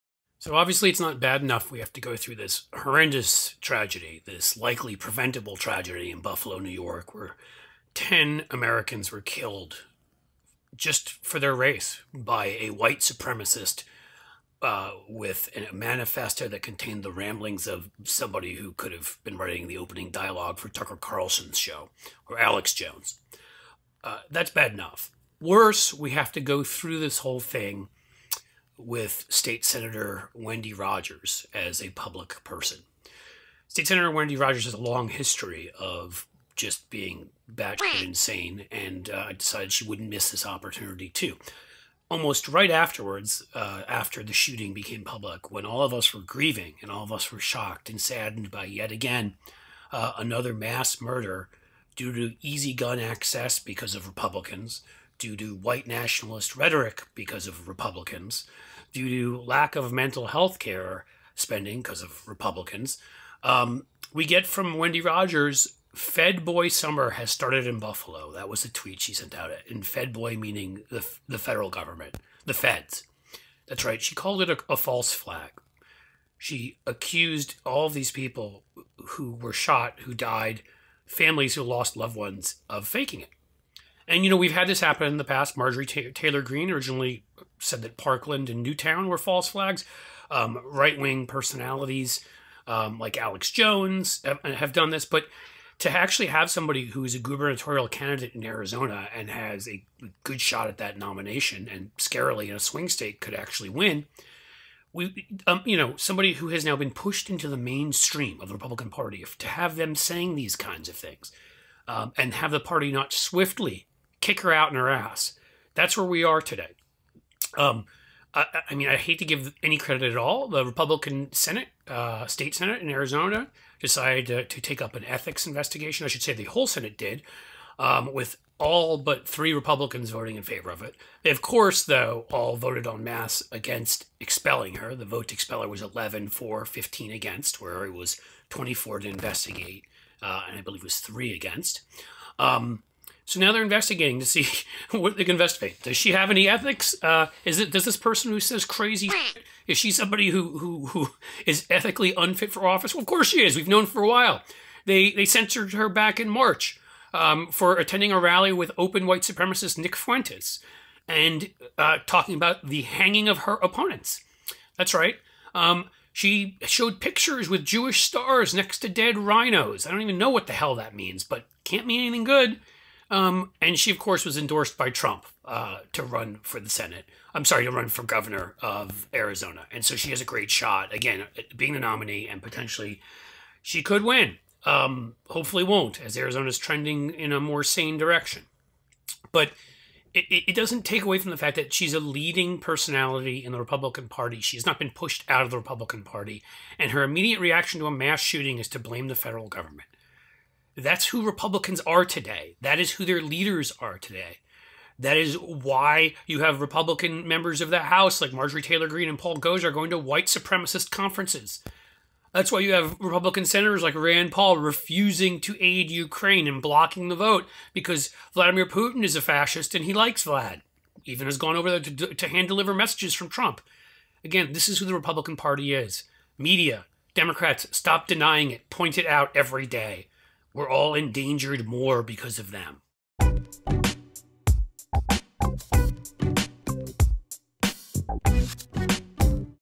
GOP Gov Candidate Wendy Rogers Calls Buffalo Attack False Flag
Wendy Rogers, State Senator & gubernatorial candidate in AZ, has called for Dems to be hanged, spent time w/ white supremacist leader Nick Fuentes and now has insulted Buffalo families still grieving by calling the market attack a false flag. She's a sickness, one that pervades the Republican Party. Cliff provides the breakdown and takedown.

**transcript generated by third-party transcription software, please excuse any typos**

so obviously it's not bad enough we have to go through this horrendous tragedy this likely preventable tragedy in buffalo new york where 10 americans were killed just for their race by a white supremacist uh with a manifesto that contained the ramblings of somebody who could have been writing the opening dialogue for tucker carlson's show or alex jones uh that's bad enough worse we have to go through this whole thing with state senator wendy rogers as a public person state senator wendy rogers has a long history of just being insane and i decided she wouldn't miss this opportunity too almost right afterwards after the shooting became public when all of us were grieving and all of us were shocked and saddened by yet again another mass murder due to easy gun access because of republicans due to white nationalist rhetoric because of republicans due to lack of mental health care spending because of republicans um we get from wendy rogers fed boy summer has started in buffalo that was a tweet she sent out it in fed boy meaning the federal government the feds that's right she called it a false flag she accused all of these people who were shot who died families who lost loved ones of faking it and you know we've had this happen in the past marjorie taylor green originally said that parkland and newtown were false flags um right-wing personalities um like alex jones have done this but to actually have somebody who's a gubernatorial candidate in arizona and has a good shot at that nomination and scarily in a swing state could actually win we um you know somebody who has now been pushed into the mainstream of the republican party if to have them saying these kinds of things uh and have the party not swiftly kick her out in her ass that's where we are today um i i mean i hate to give any credit at all the republican senate uh state senate in arizona decided to take up an ethics investigation i should say the whole senate did um with all but three republicans voting in favor of it they of course though all voted on mass against expelling her the vote expeller was 11 4 15 against where he was 24 to investigate uh and i believe was three against um so now they're investigating to see what they can investigate does she have any ethics uh is it does this person who says crazy is she somebody who who is ethically unfit for office well of course she is we've known for a while they they censored her back in march um for attending a rally with open white supremacist nick fuentes and uh talking about the hanging of her opponents that's right um she showed pictures with jewish stars next to dead rhinos i don't even know what the hell that means but can't mean anything good um and she of course was endorsed by trump uh to run for the senate i'm sorry to run for governor of arizona and so she has a great shot again being the nominee and potentially she could win um hopefully won't as arizona is trending in a more sane direction but it doesn't take away from the fact that she's a leading personality in the republican party she's not been pushed out of the republican party and her immediate reaction to a mass shooting is to blame the federal government that's who republicans are today that is who their leaders are today that is why you have republican members of the house like marjory taylor green and paul goes are going to white supremacist conferences that's why you have republican senators like rand paul refusing to aid ukraine and blocking the vote because vladimir putin is a fascist and he likes vlad even has gone over to hand deliver messages from trump again this is who the republican party is media democrats stop denying it point it out every day we're all endangered more because of them